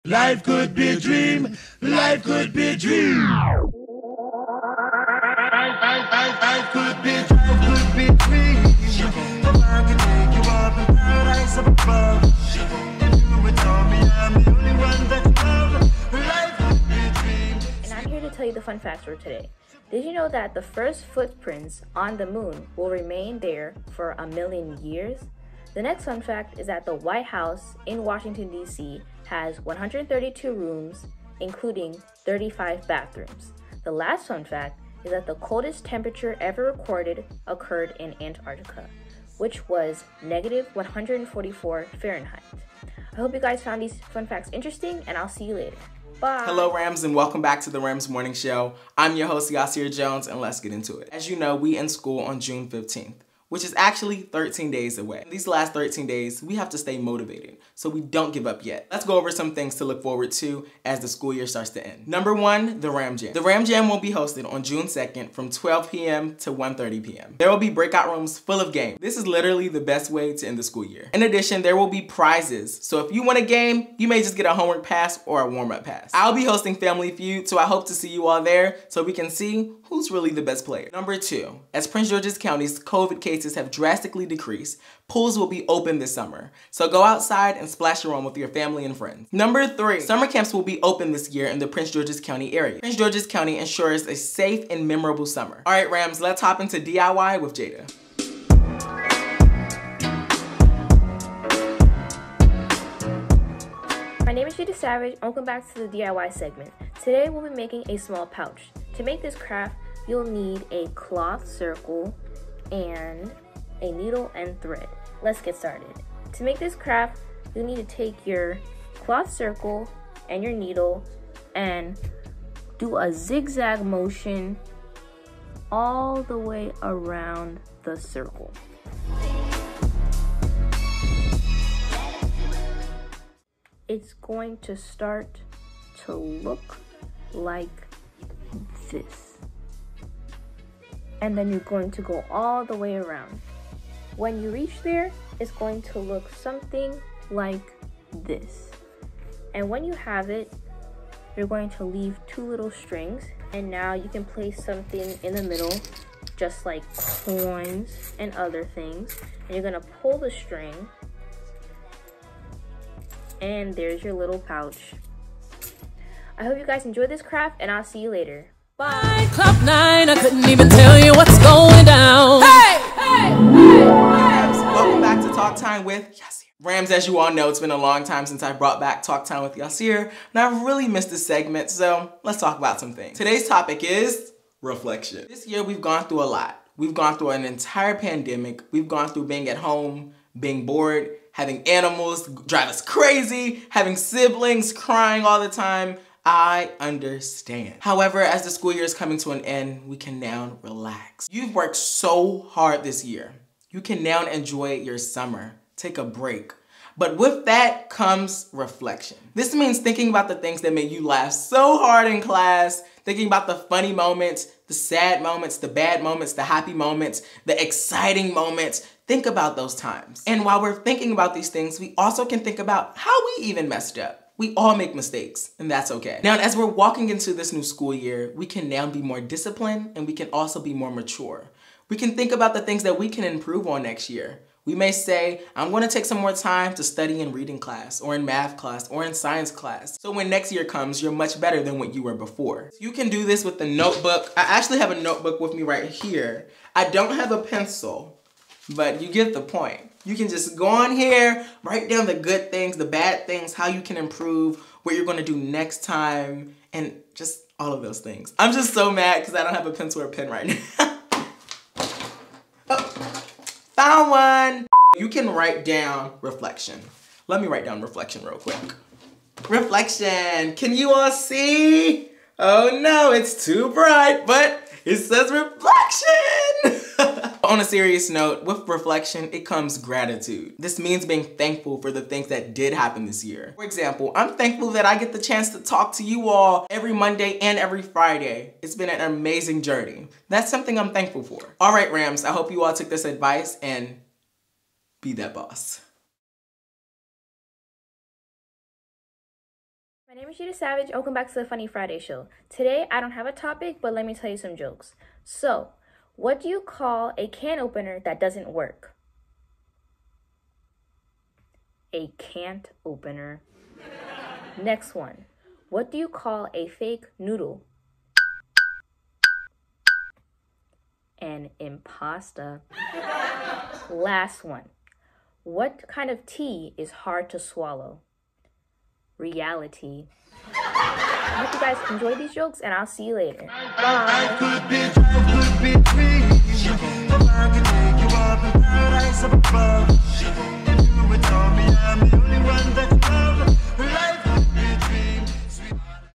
THAT Tutorial! | Season 4 EP7
Happy Friday :)

📝 Notes:
The Ram Jam will be on June 2nd (next Wednesday), 2021.
Submit a video and have it be shown on the Ram Jam!

📝 Yasir was very happy to bring back “Talk Time with Yasir”. Now returning he wants students to reflect on their school year, allowing them to improve for a new level next year.

🎨 Jada gets crafty and makes a pouch to carry coins or other items DIY style!

Rams enjoy your weekend.
-TRMS